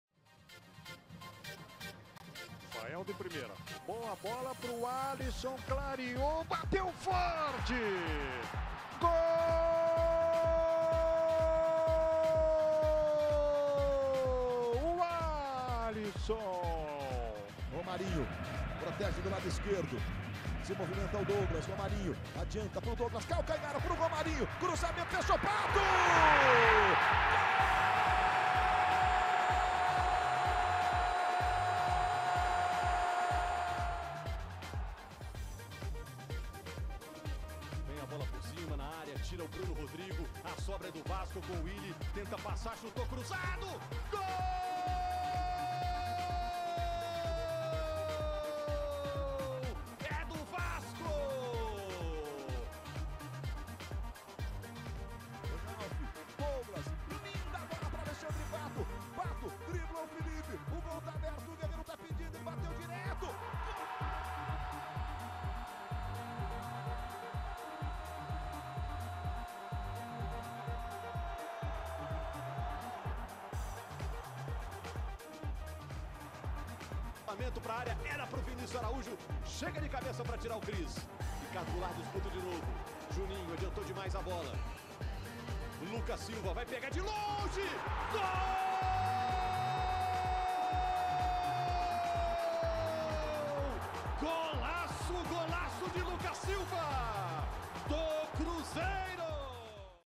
Top 5 goals of the week - Campeonato Brasileiro
5) Wallyson (Bahia, 24, Brazil) in Portuguesa 4 Bahia 2
4) Alexandre Pato (Corinthians, 24, Brazil) in Corinthians 4 Flamengo 0
3) Willie (Vasco da Gama, 20, Brazil) in Cruzeiro 5 Vasco da Gama 3
2) Alexandre Pato (Corinthians, 24, Brazil) in Corinthians 4 Flamengo
1) Lucas Silva (Cruzeiro, 20, Brazil) in Cruzeiro 5 Vasco da Gama 3